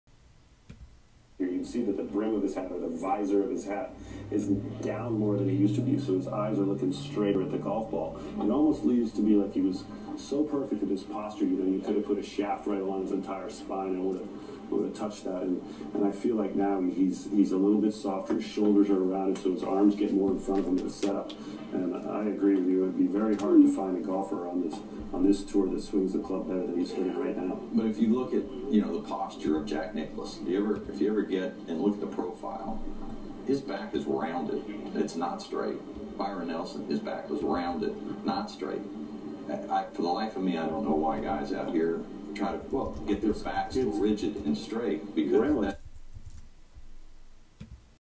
Adam Scott's Posture (Chamblee and Faxon)
Brad Faxon and Brandel Chamblee discuss Adam Scott's new and improved setup position the past few years.